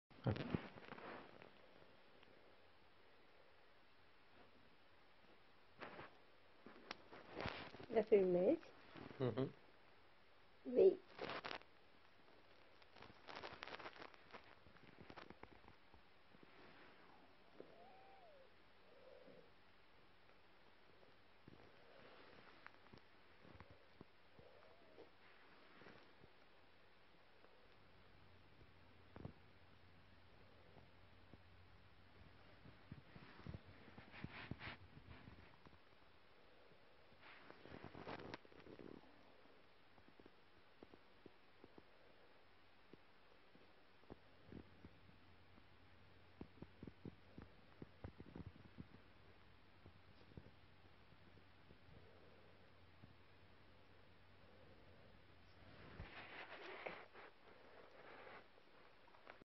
Guinea pig likes to be scratched
Guinea pig likes to be scratched before going to sleep